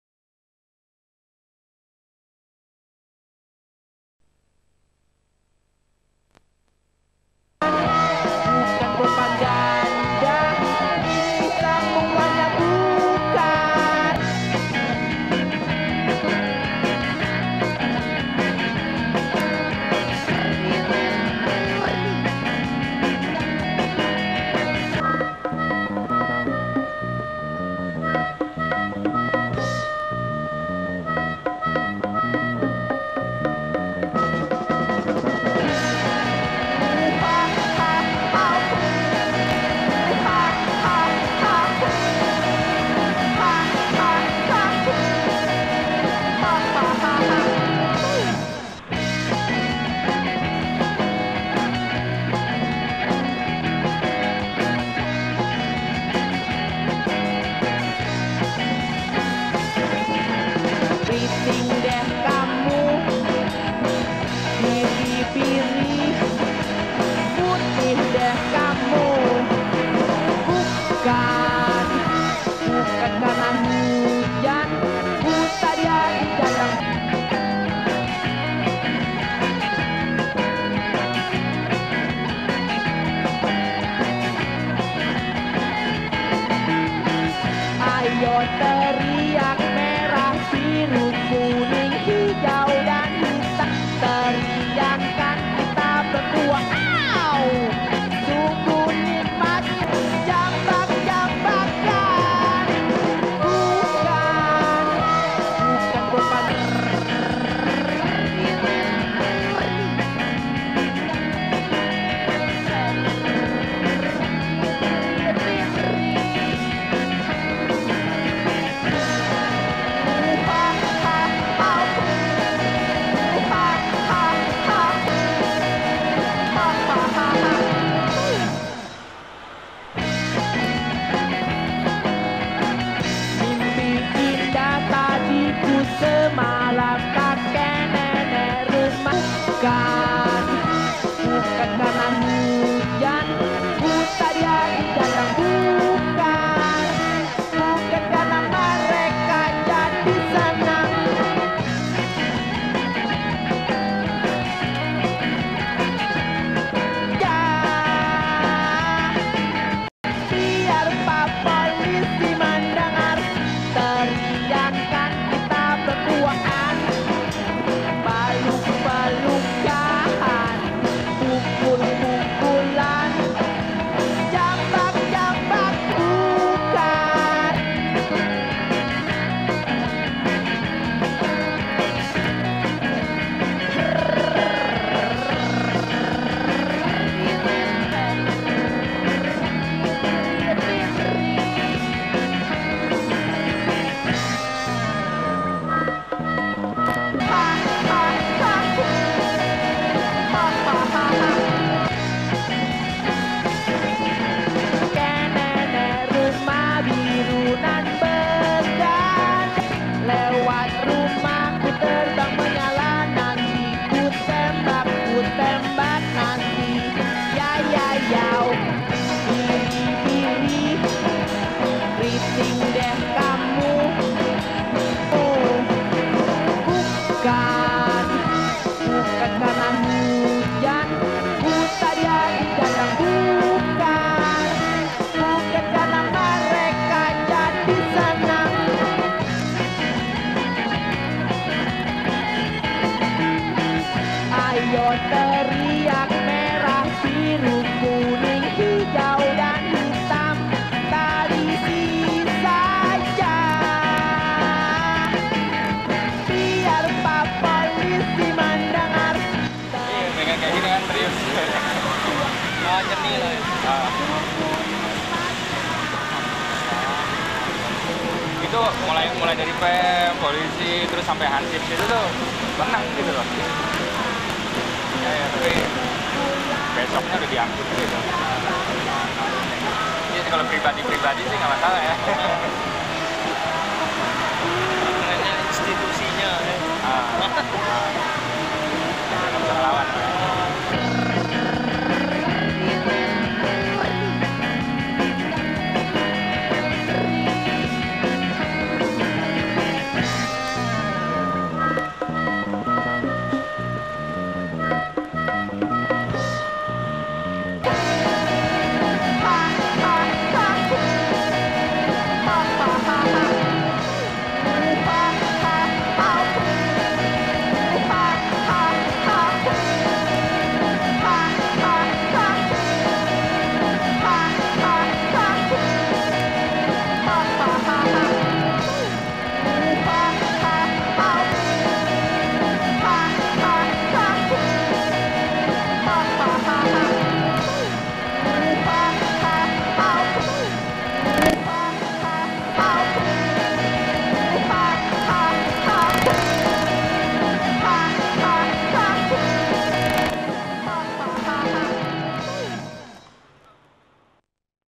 IVAAarchive | JAKARTA HABITUS PUBLIK JakArt
Jakarta Habitus Publik merupakan sebuah proyek seni yang menjadi bagian dari JakArt tahun 2001 dan sekaligus menjadi rangkaian acara Pekan Raya Jakarta (PRJ) 2001. Jakarta Habitus Publik menampilkan aktivitas seni di ruang publik. Para seniman yang terlibat dalam proyek ini kemudian merespon ruang publik semisal halte bus dekat Plaza Indonesia atau Terminal bus Blok M dengan mural dan instalasi.